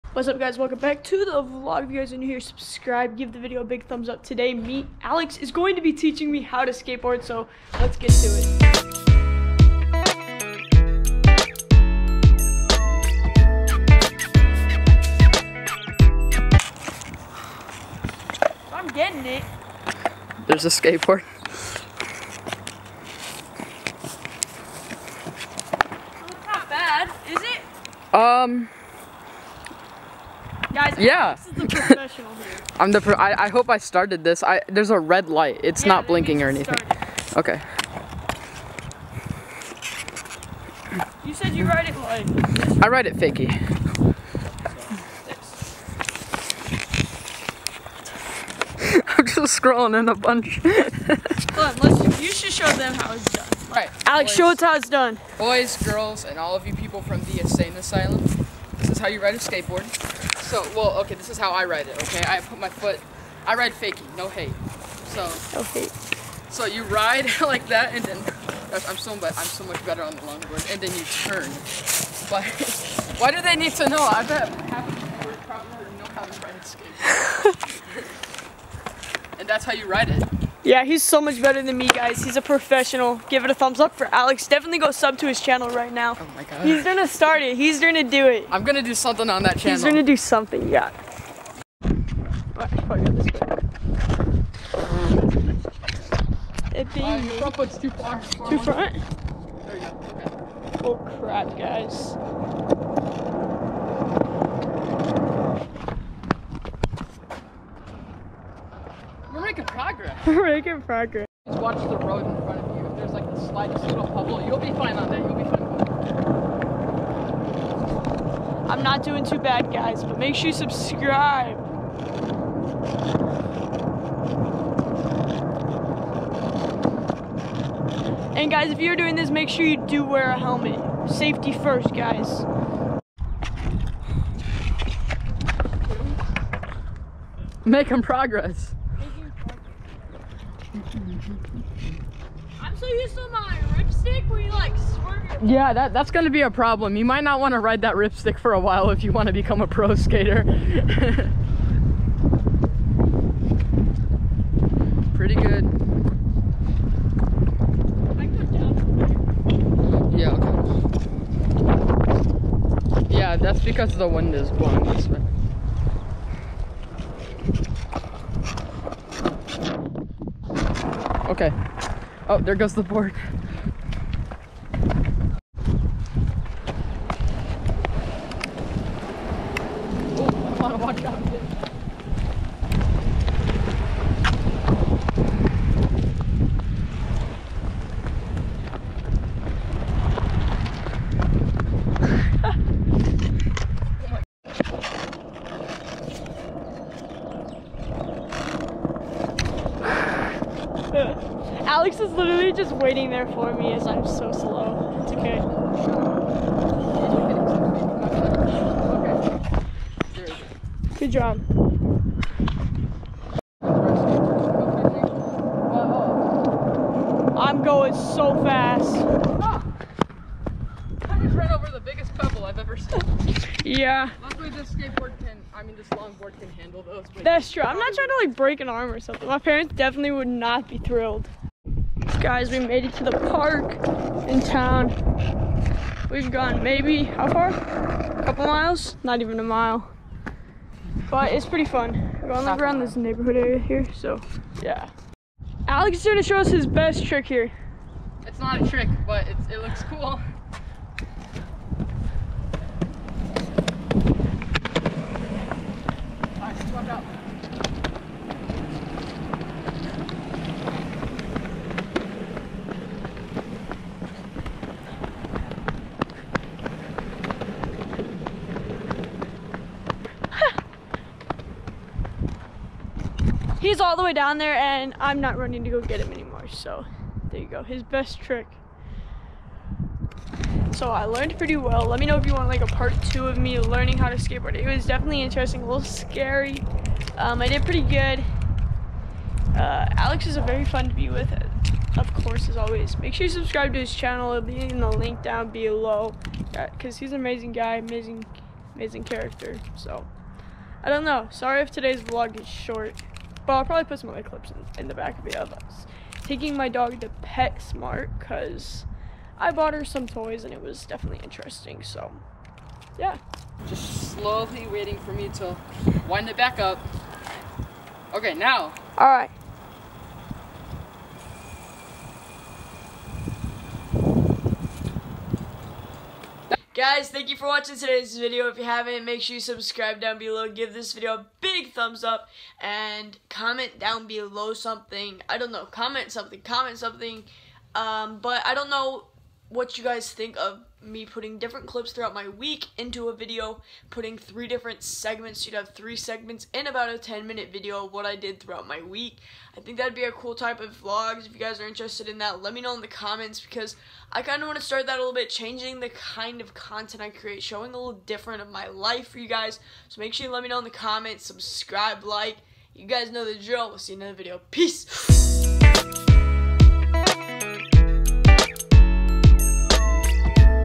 My Best Friend Teaches Me To Skateboard
Welcome Back To The Mitchell Life!!
Today my best friend Alex taught me how to skateboard and it was so much fun!! I may not be very good at it yet but over time I will get better!!
-Mitch

Alex's Channel: @itsalex238

Comment "Mitch" if you saw this :)